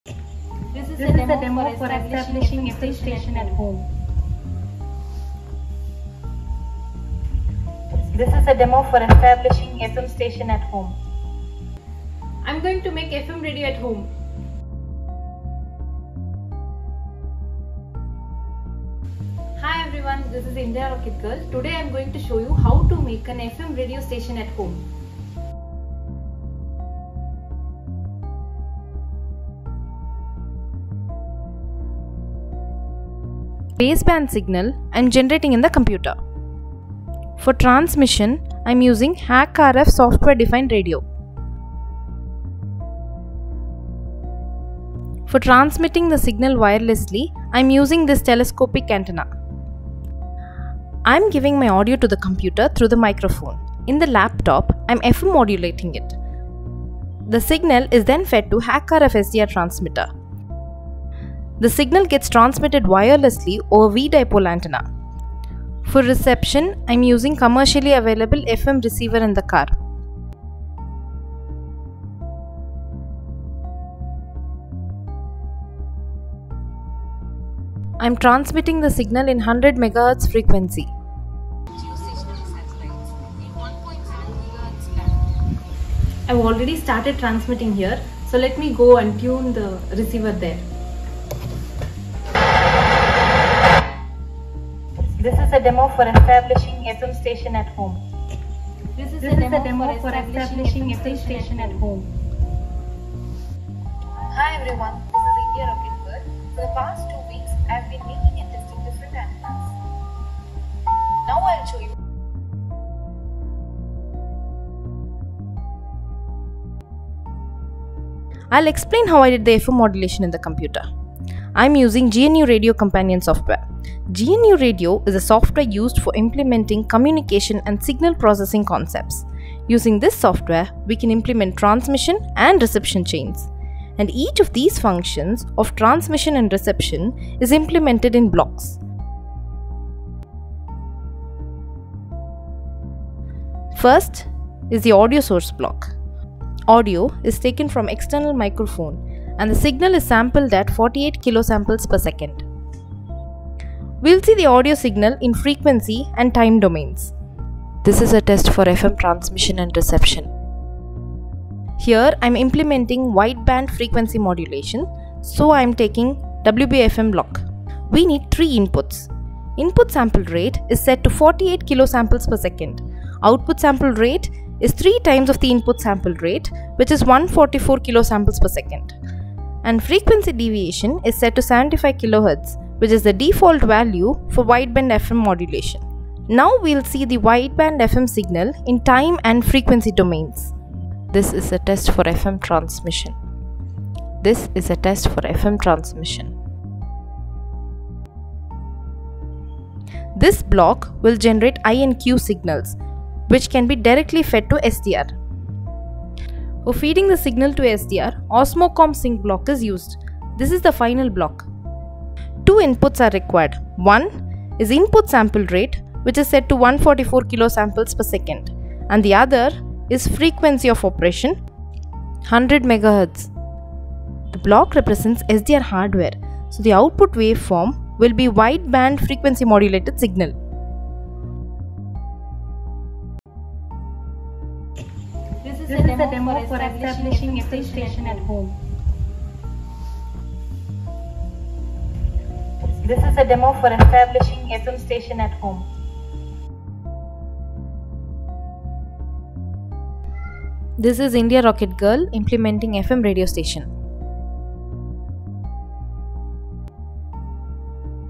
How to Make FM Radio Transmission @ Home | IndiaRocketGirl
In this video I have explained how to make FM Radio station using Software Defined Radio.

Time Stamps
00:00 Introduction
00:25 Transmitter Setup
02:00 GNU Radio
03:50 Final Result
04:06 Conclusion